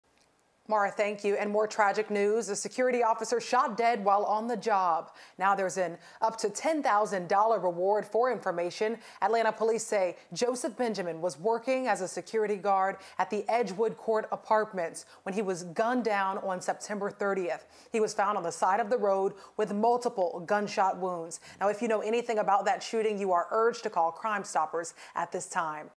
Security officer shot dead while on the job at Edgewood Court Apartments
He was gunned down o Sept. 30.